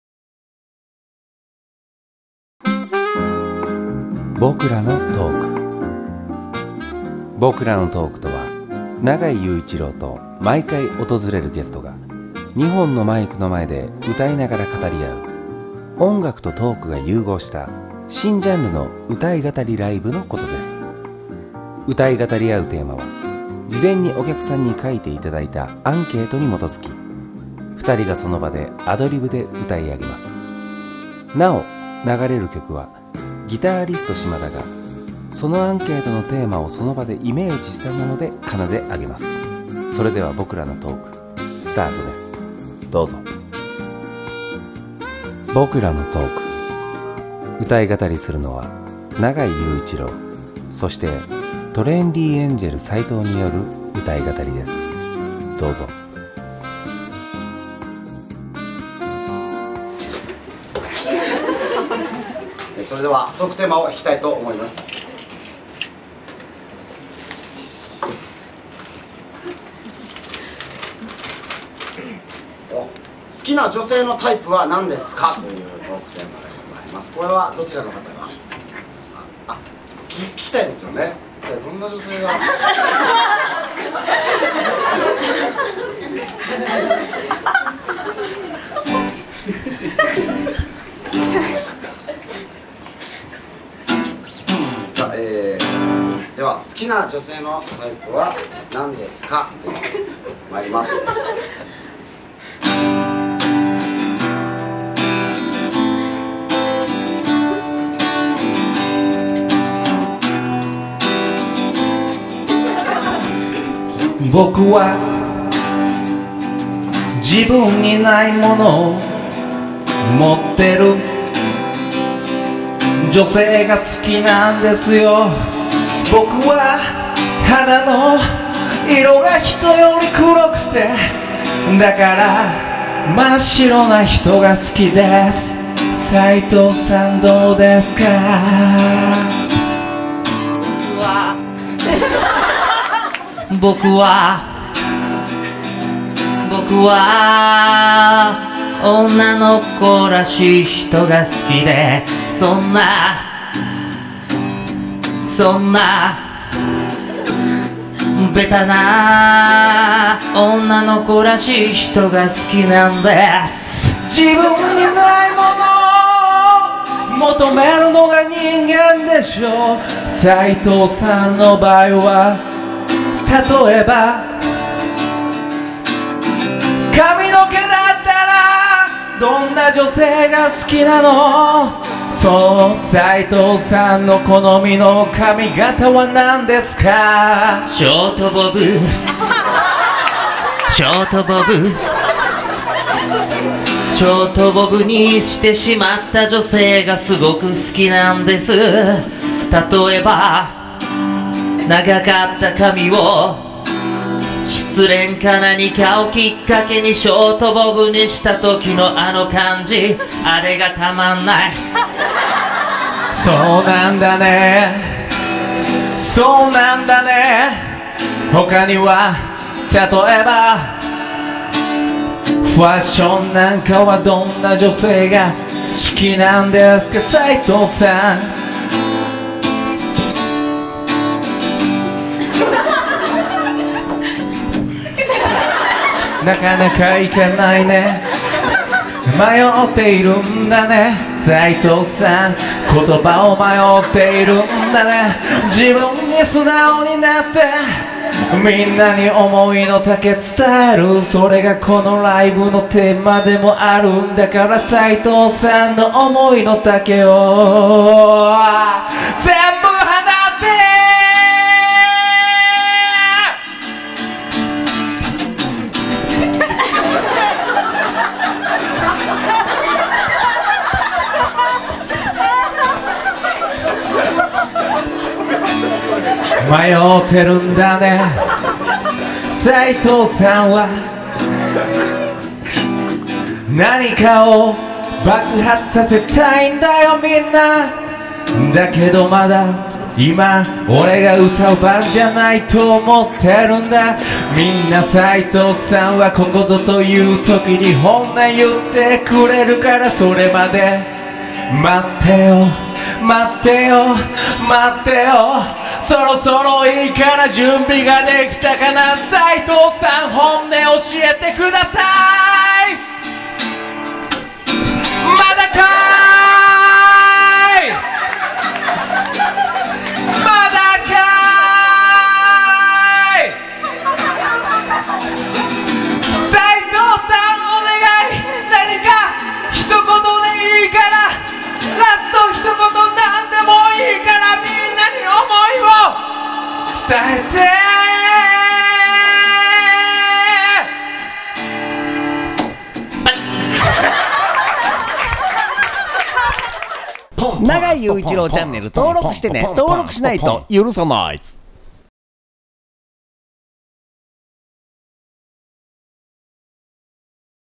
トレンディエンジェル斉藤×永井佑一郎【僕らのトーク】♯1
永井佑一郎presents

【僕らのトーク】vol.Ⅲ♪スペシャルトーク♪

11/9

【歌い語り】

♪どりあんず平井♪グランジ遠山♪こがけん♪エリートヤンキー橘♪しずる池田♪ジューシーズ児玉♪ペペ岡部♪ボーイフレンド宮川♪ラフレクラン西村♪トレンディエンジェル斎藤

【サックス】西村ヒロチョ
【ギター】シマッシュレコード嶋田
【ドラム】ビスケッティ岩橋

【プロデューサー】永井佑一郎